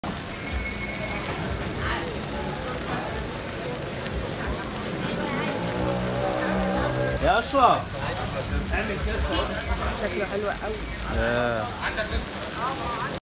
Worlds best street food | 🐟 Incredible Grilled Seafood
Some of the world best street food, a grilled seafood feast of flavors. In Egypt’s Ras El bar a small fish en village on the Mediterranean you can find some of the best fish in Egypt. A wide variety of everything from the surrounding southern Mediterranean fishing grounds. Ras el bar is located where the Nile river meets the Mediterranean Sea this helps add even more to the variety of fish available. Here you can find the seafood prepared in many different ways.  Grilled,  fried,  boiled,  full of different herbs and spices  and even baked in a  wood fire oven adding a delicious smokey flavor to the food. Ras el bars fishmarket has so much great seafood to choose from.  The water is very rich and the fish market at Ras el Bar has so much to offer like Striped sea bass, porgy, squid, shrimp, clams, flounder, grouper, red mullet, tuna, and so much more. It’s a great little town for incredibly delicious seafood.